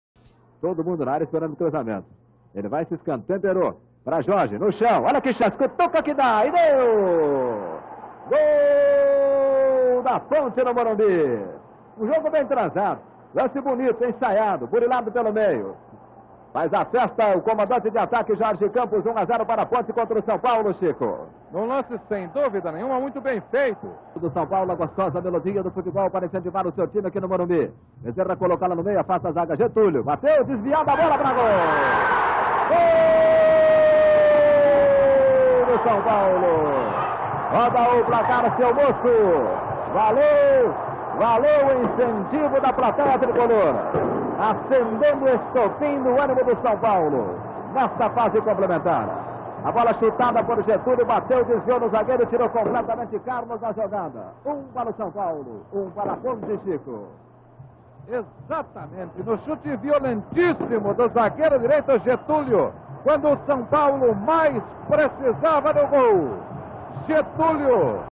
São Paulo 1 x 1 Ponte Preta (Campeonato Paulista 1979)
Gols da partida realizada no Estádio do Morumbi, em São Paulo (SP).

Imagens: TV Bandeirantes
Narração: Alexandre Santos
Reportagem: Chico de Assis